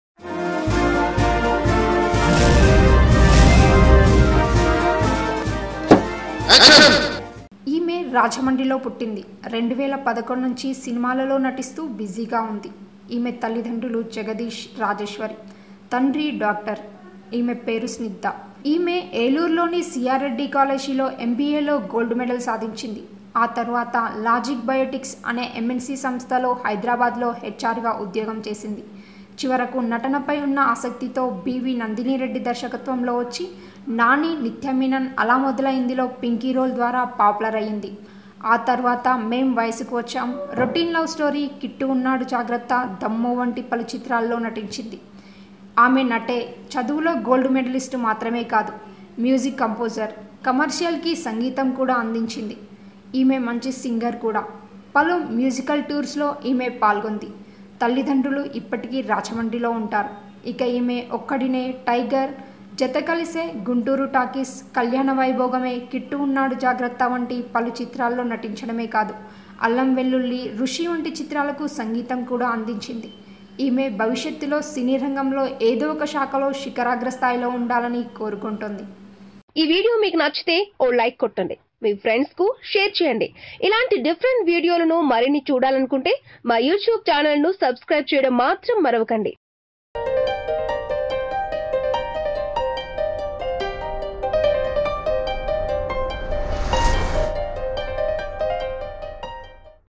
Ala Modalaindi Fame Snigdha Career & Job | Telugu Artist Snigdha Personal Life | Tollywood Today
Ala Modalaindi Fame Snigdha Career & Job... Telugu Artist Comedy Roles Artist Snigdha Personal Life can shock you. Check out this full video to know her details.